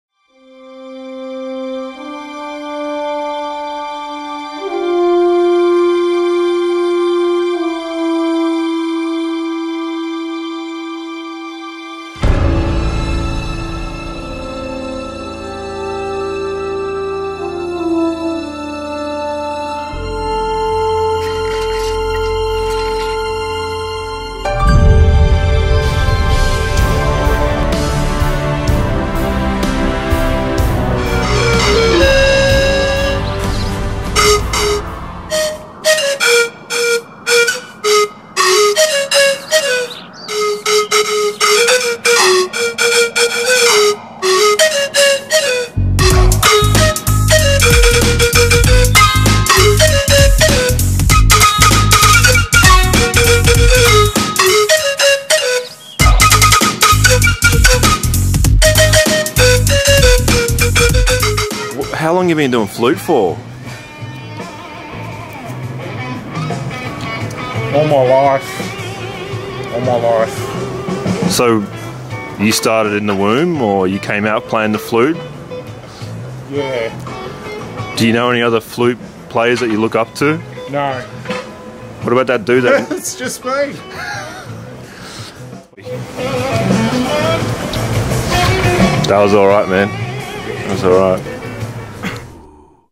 junk yard flute - created and played by John Penn. must watch to believe.
there's a moment in everyones life, amongst the tough times, when all seems  lost, bogged down in the grind of the mundane... there's a moment when hope rises, and we find that junkyard flute.

this short film is dedicated to those that know